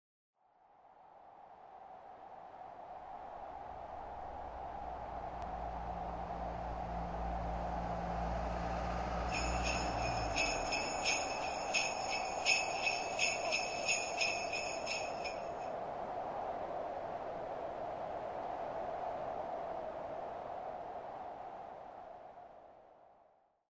Santa test drives the sleigh over Westport House
Santa and the reindeer were spotted test driving the sleigh over Westport House today in preparation for their big journey tonight. They'll be leaving the House shortly to visit all the good boys & girls. Make sure you get to bed early tonight to wake up to all his wonderful surprises!